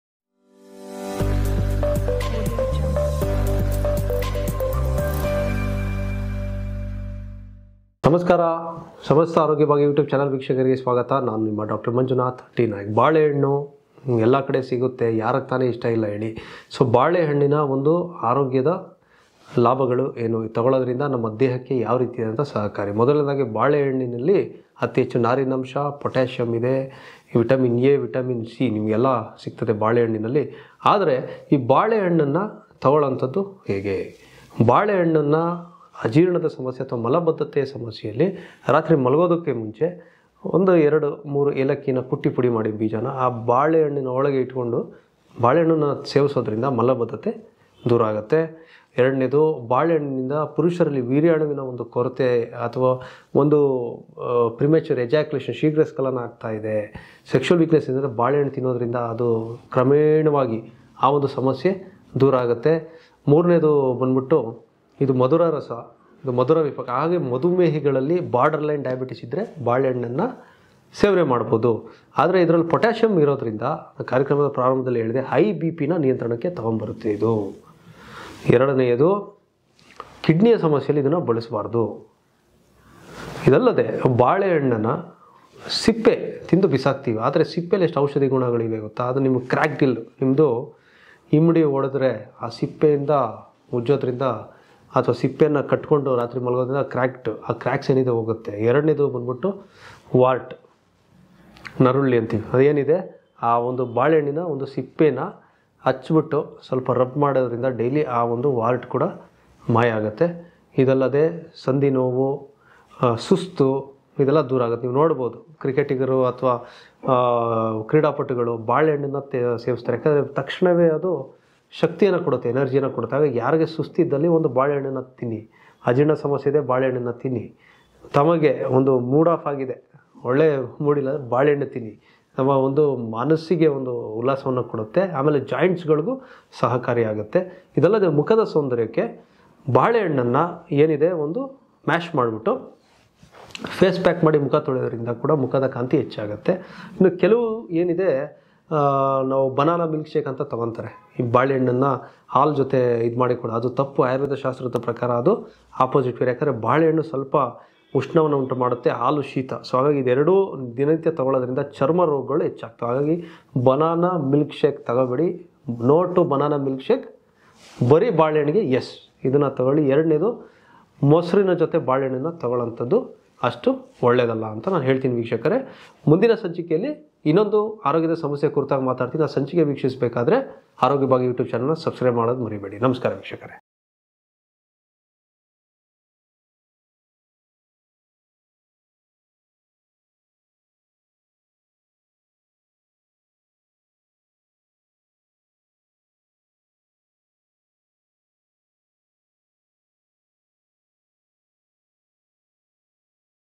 ದಿನಾಲೂ1 ಬಾಳೆಹಣ್ಣು ತಿಂದು ನೋಡಿ ಚಮತ್ಕಾರ..! | Banana Health Benefits In Kannada | Arogya Bhagya Kannada
Dr Manjnath D Naik
Ayurveda & Nadi Specialist
Dheerghayush Ayurveda Clinic
Bangalore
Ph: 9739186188